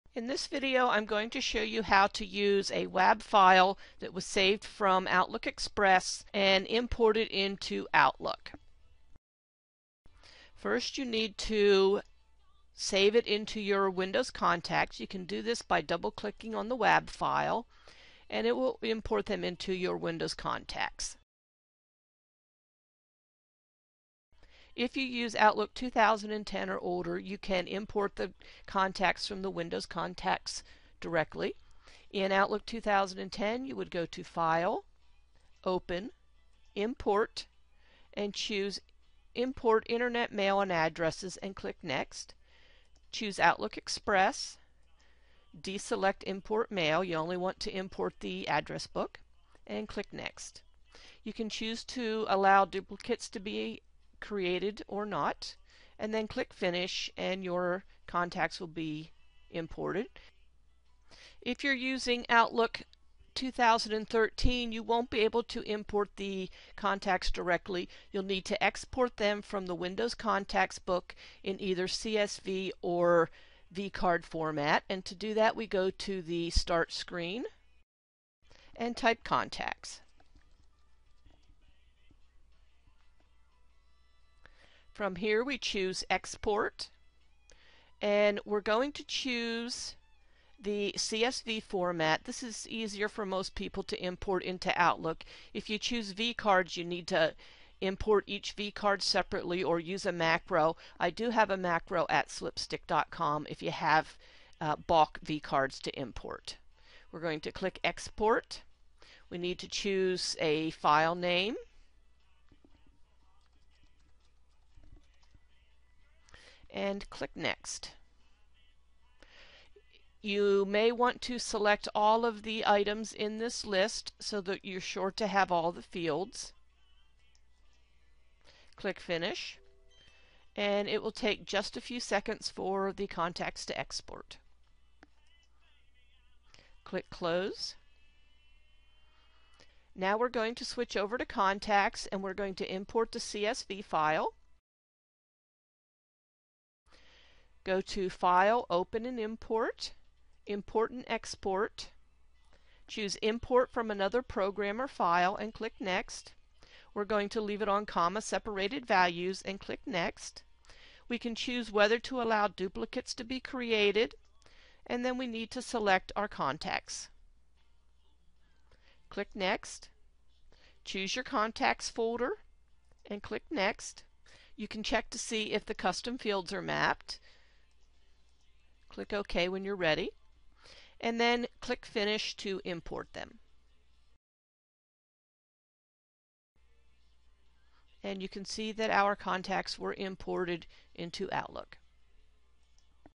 Import .wab into Outlook on Windows 7 or 8
How to import a wab (Windows Address Book) into Outlook installed on Windows 7 or Windows 8